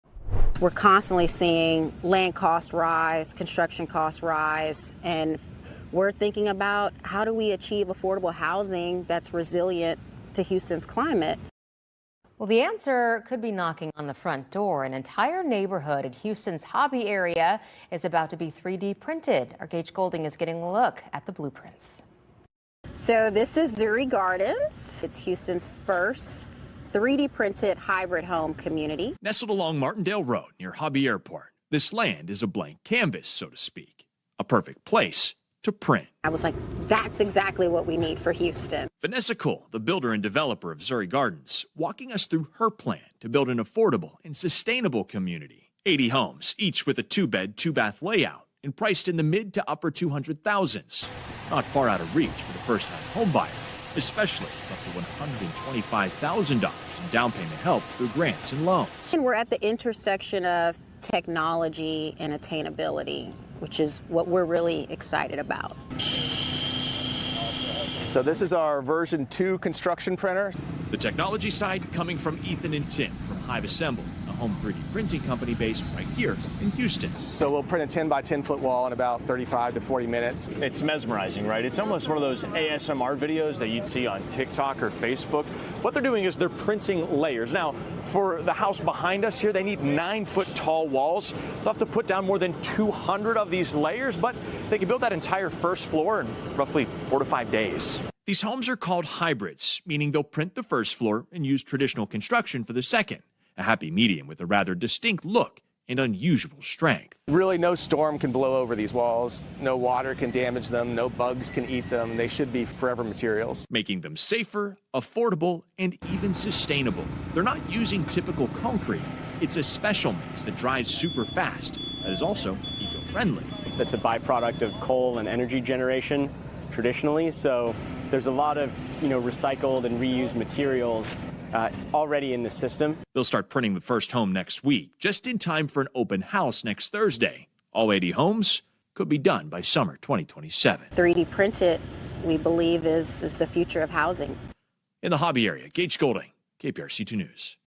Houston’s first 3D printed home community is almost ready to roll or print
Houston's first 3D-printed hybrid home community, Zuri Gardens, is nearing completion along Martindale Road near Hobby Airport. Vanessa Cole, the developer, plans to build 80 affordable homes with two bedrooms and two bathrooms, priced between $200,000 and $250,000, with up to $125,000 in down payment assistance available. The project uses a construction printer to print the first floor in four to five days, combining 3D printing with traditional construction for the second floor. The homes feature resilient, eco-friendly materials, and the entire community is expected to be completed by summer 2027.